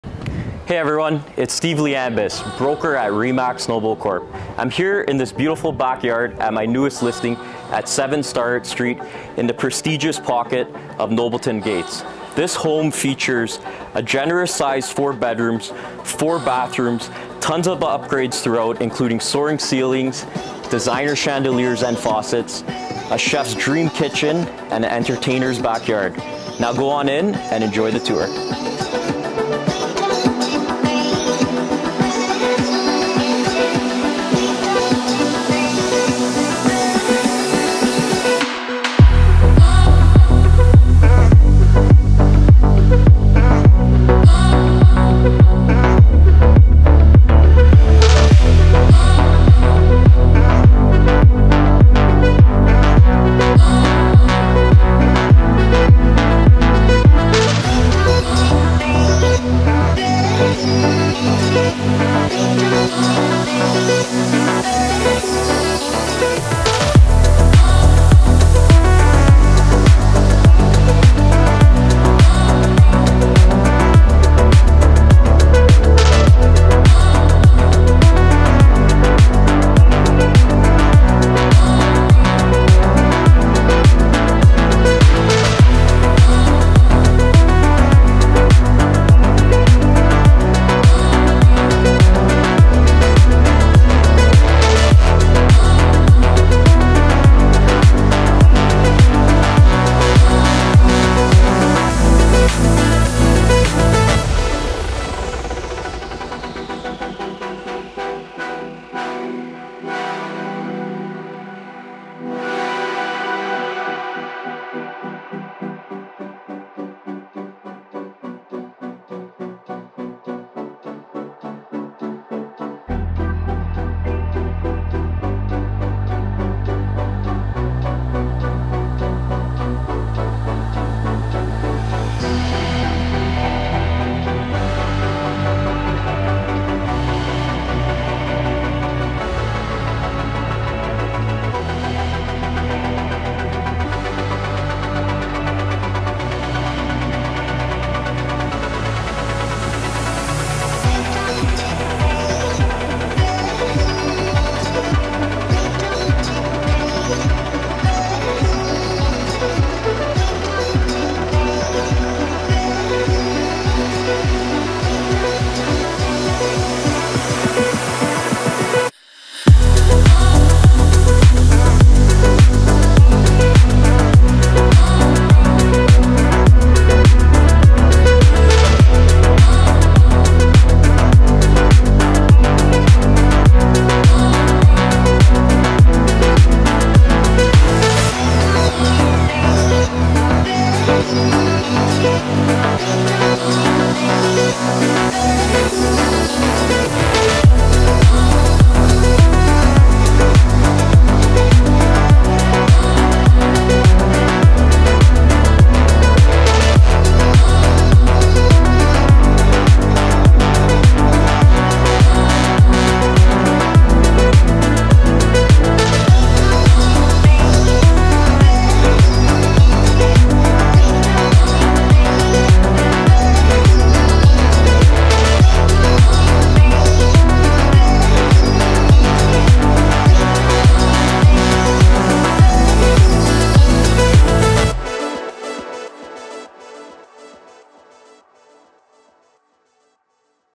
7 Starrett Street | Nobleton Ontario Canada | Gates of Nobleton
Introducing a captivating residence nestled in the prestigious Nobleton Gates community. This remarkable upgraded home presents an exquisite blend of luxury and comfort. Boasting 4 spacious bedrooms and 4 modern bathrooms, this property offers ample space for the whole family to unwind and relax.

Step into the heart of this home, and you'll find an entertainer's dream backyard, perfect for hosting gatherings and creating lasting memories. Immerse yourself in the tranquility of the beautifully landscaped outdoor space, featuring an oversized pergola with ample seating space and a fireplace for roasting marshmallows.

Inside, the chef kitchen awaits, designed to inspire culinary creations. Every detail has been thoughtfully considered, from the high-end appliances and facuets, to the abundant storage and countertop space. It's a haven for those who appreciate both style and functionality.

This home in the Nobleton Gates community offers not only a luxurious living space but also access to a range of amenities. Enjoy the serenity of the neighbourhood, with its tree-lined streets and exclusive atmosphere. Nearby, you'll find, parks, golf courses, recreations centres and much more.

With its upgraded features, stunning backyard, and chef kitchen, this property is an exceptional find. Don't miss the opportunity to call this prestigious address your own. Schedule a viewing today and experience the epitome of luxury living in the Nobleton Gates community.